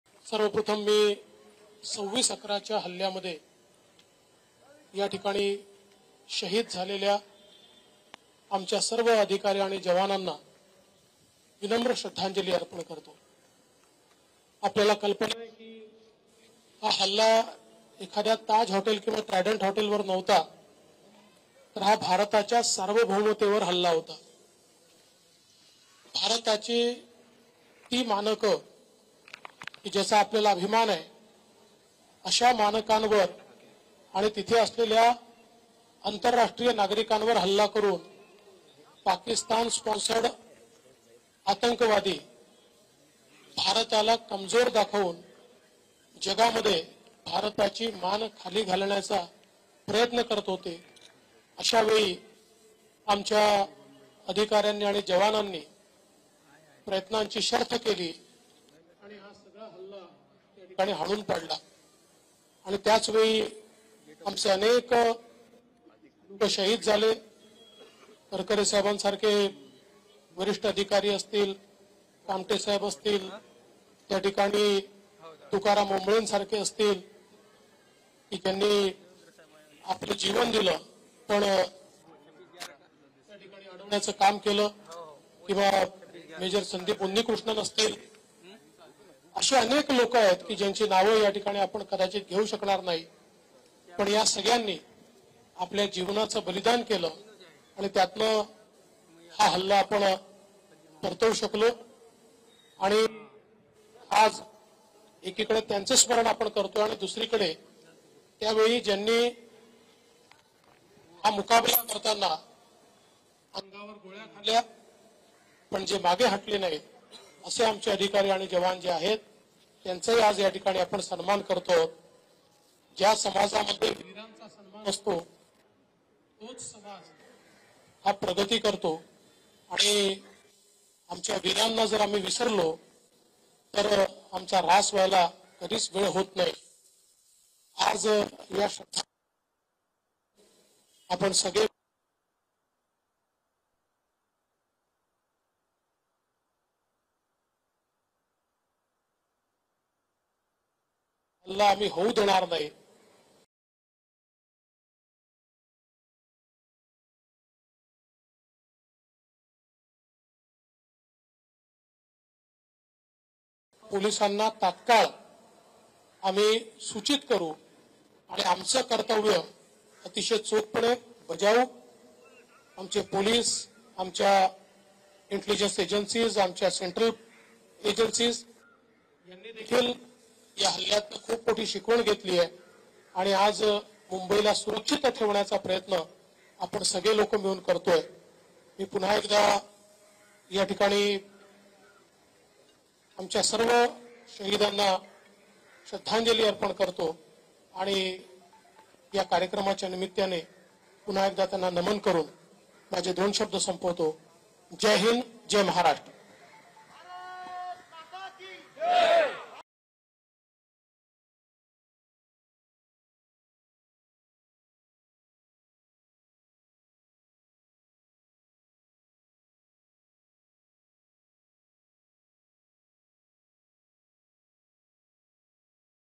Devendra Fadanvis : फडणवीसांनी जागवल्या मुंबईतील दहशतवादी हल्ल्याच्या आठवणी...|LetsUpp Marathi
26/11 दहशतवादी हल्ल्यातील जखमी पोलीस अधिकाऱ्यांचा सन्मान केल्यानंतर देवेंद्र फडणवीसांनी मुंबईतील दहशतवादी हल्ल्याच्या आठवणी जागवल्या.